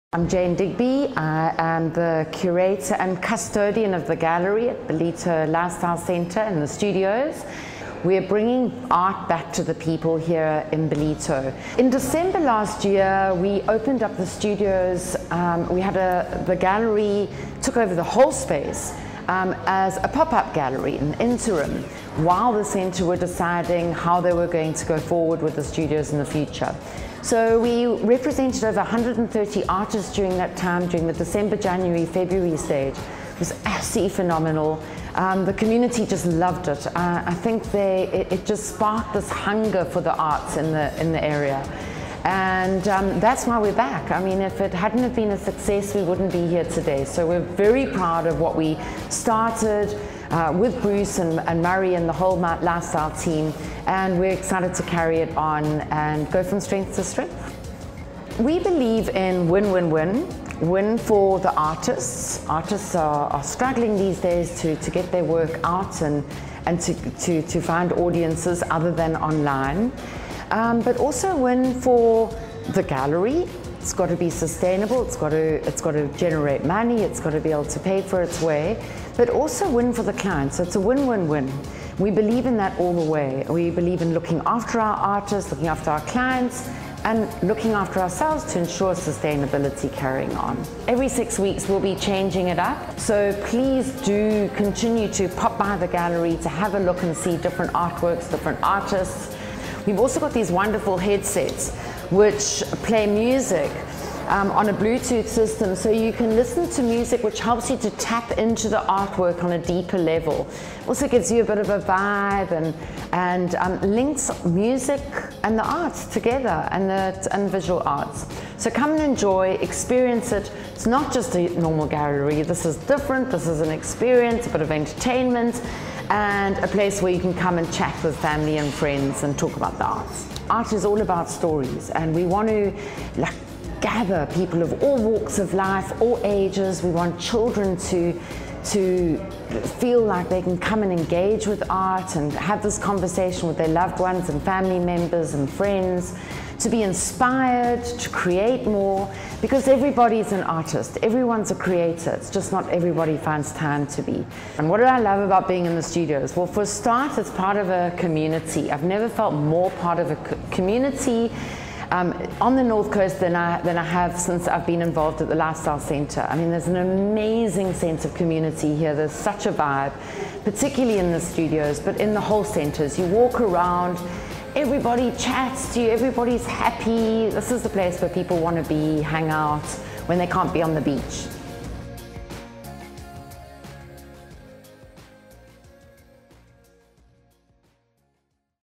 The Gallery
Following the success of the pop up concept over the 2020/21 season, The Gallery returns to deliver an infusion of culture to The Studios. Curated and managed by local artist and oil painter, Jane Digby, a range of art and design will be showcased for art lovers or browsers in a unique space within The Studios, including paintings, drawings, print, mixed media, photography, sculpture, ceramics, woven ware and more! The Gallery provides a platform for selected local artists to showcase their talent, at the same time nurturing the enjoyment and appreciation of arts and culture on the Dolphin Coast.